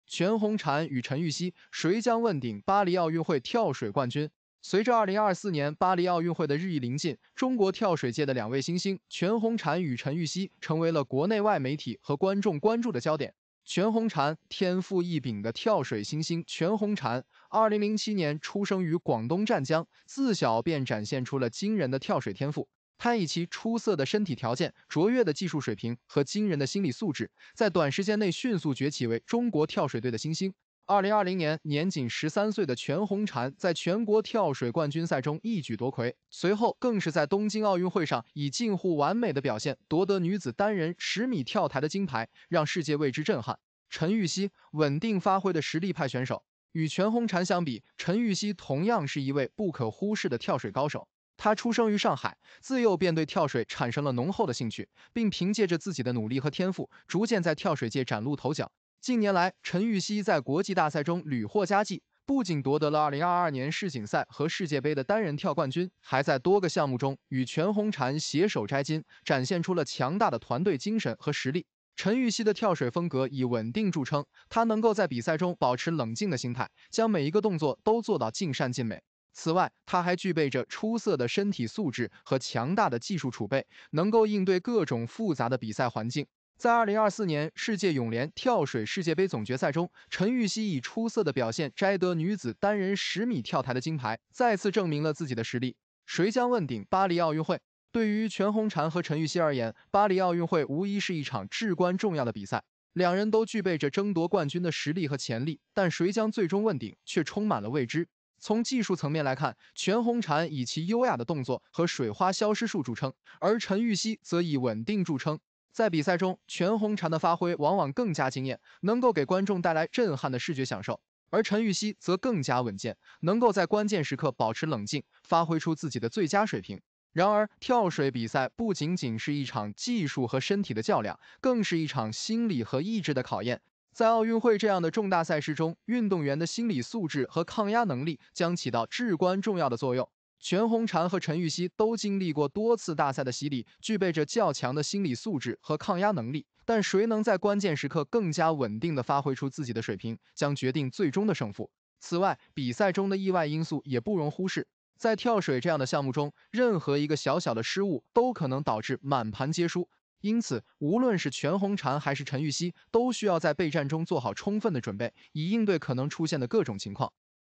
全红婵与陈芋汐：谁将问鼎巴黎奥运会跳水冠军？周继红说出人选
全红婵巴黎奥运会备战训练细节，出征巴黎的幕后故事